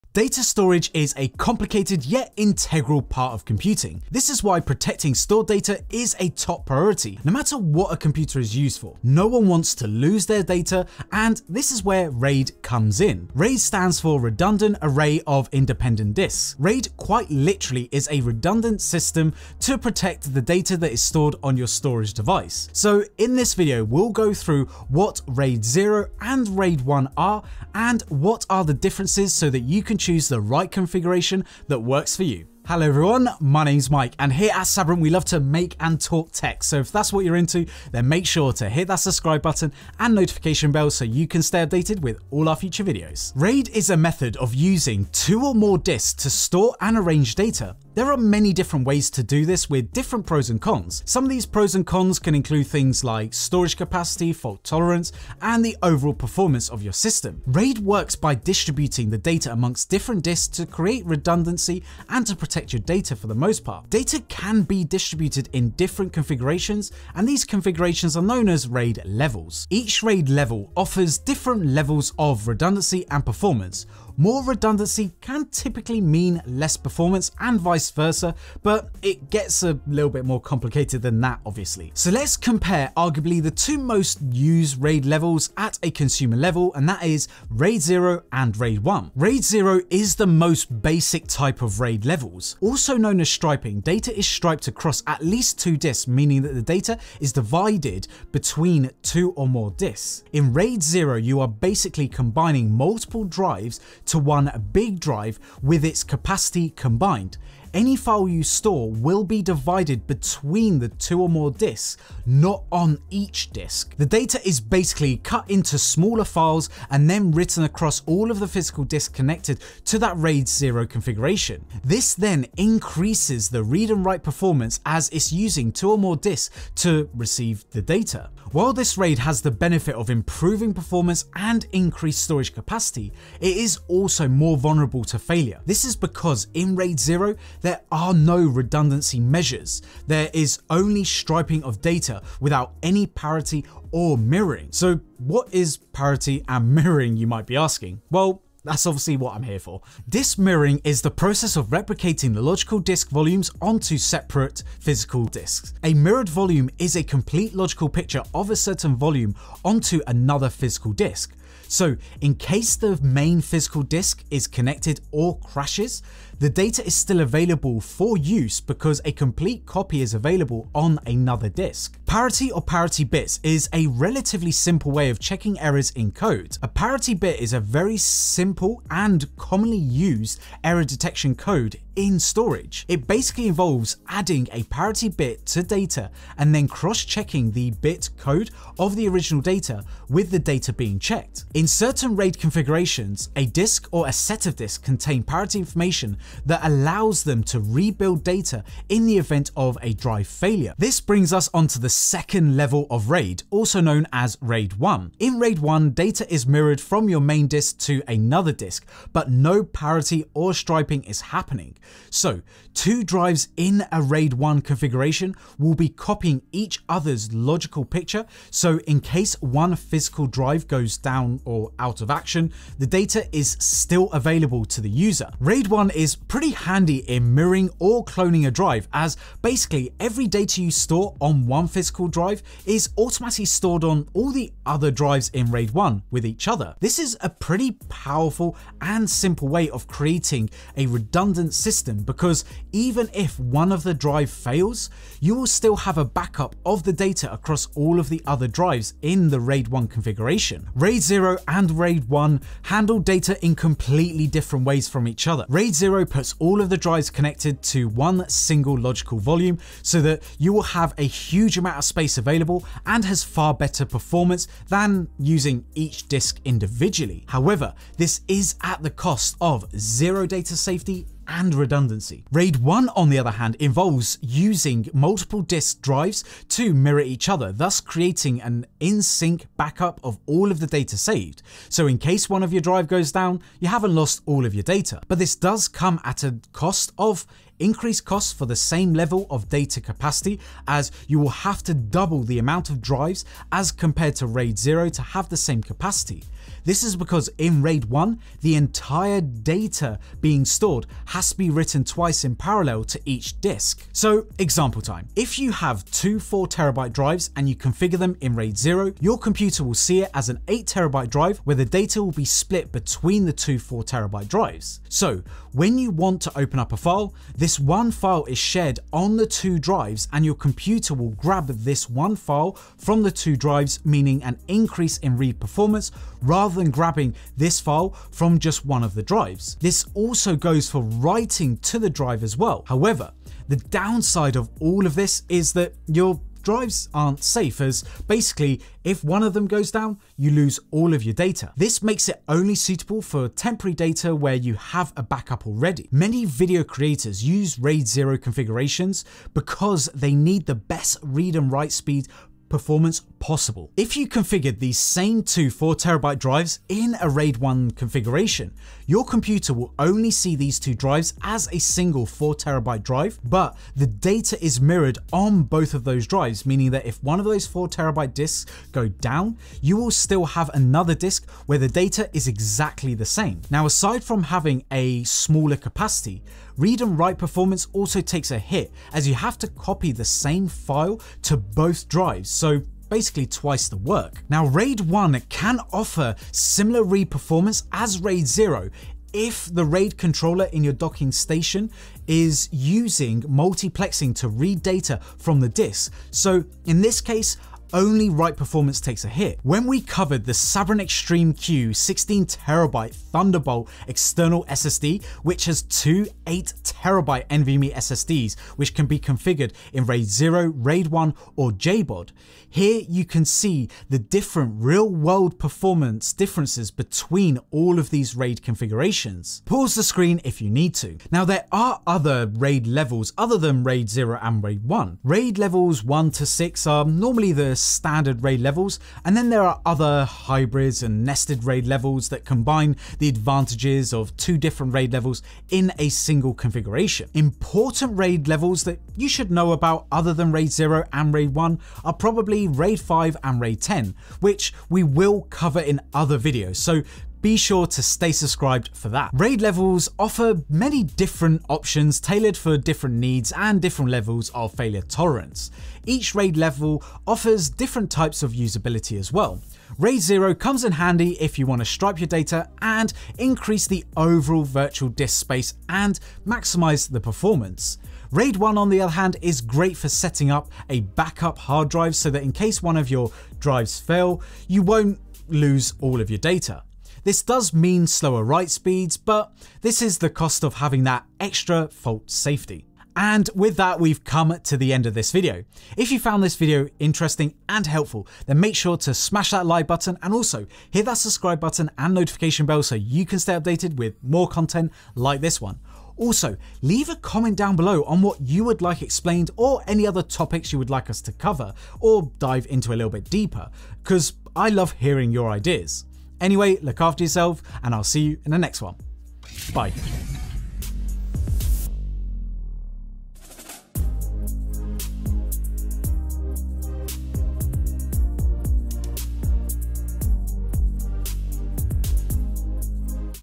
RAID0 vs RAID1 | Beginners Explanation!
Protecting your data or maximizing your storage configuration can change how you use your computer and your daily workflow! Two popular but different configurations are RAID0 and RAID1 as they are simple to set up. But what exactly are they and which should you use? Well, we cover the basics of RAID0 and RAID1 and the benefits and disadvantages of both!